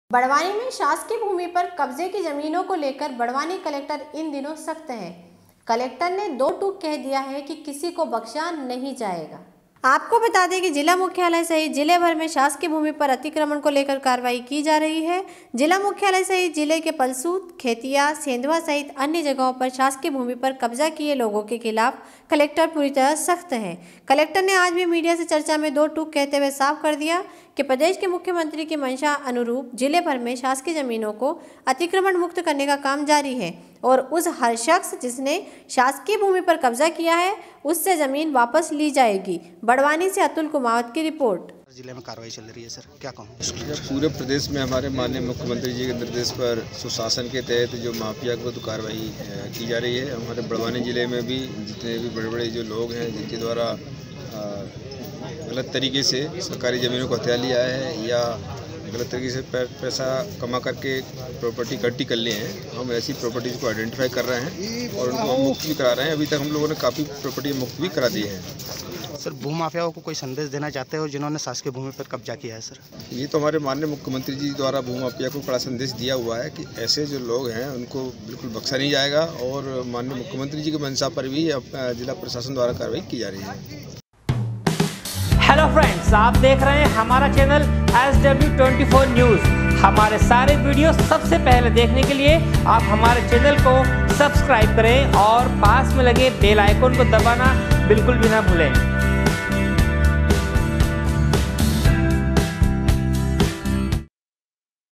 Barwani- बड़वानी में शासकीय भूमि पर कब्जे की जमीनों को लेकर बड़वानी कलेक्टर इन दिनों सख्त है।
Barwani- SW 24 News Madhyapradesh
बड़वानी में शासकीय भूमि पर कब्जे की जमीनों को लेकर बड़वानी कलेक्टर इन दिनों सख्त है। कलेक्टर ने उन्होंने दो टूक कह दिया कि किसी को बख्शा नही जाएगा। बड़वानी से अतुल कुमावत की रिपोर्ट।